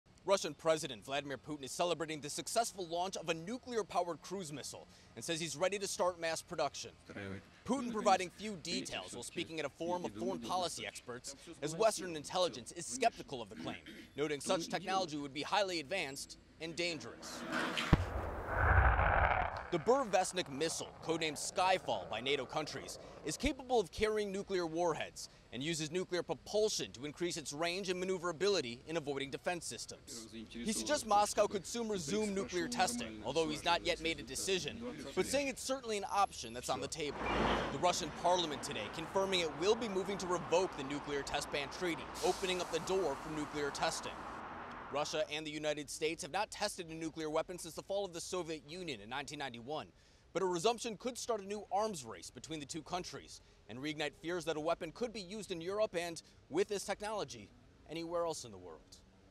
Russia Test Nuclear Missile; May Resume Testing
President Vladimir Putin claimes Russia has successfully tested the Burevestnik--an experimental nuclear-powered cruise missile--and is considering resuming nuclear testing.